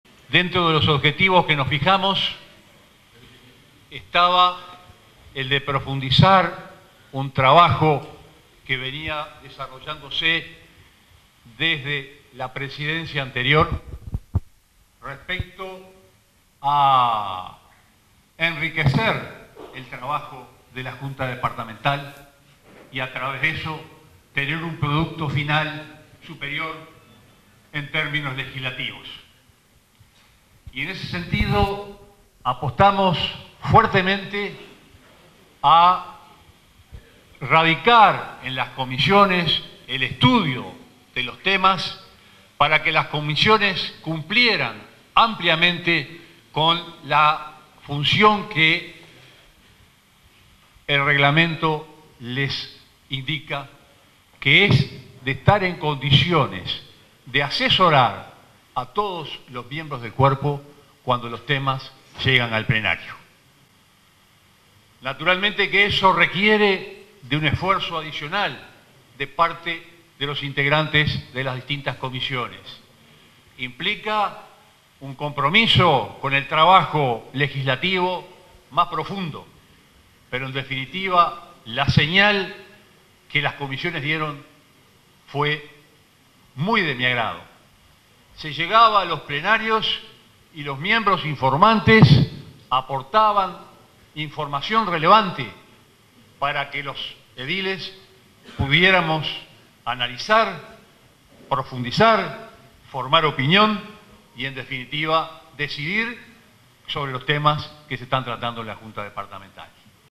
Carlos Martínez Usoz
El presidente saliente de la Junta Departamental de Florida, edil Carlos Martínez Usoz, hizo una evaluación de su gestión, puntualizando que se profundizó el enriquecimiento del trabajo de la cuerpo.
También destacó que se apostó a radicar en las comisiones el estudio de los temas, existiendo “aporte de información relevante en los plenarios”.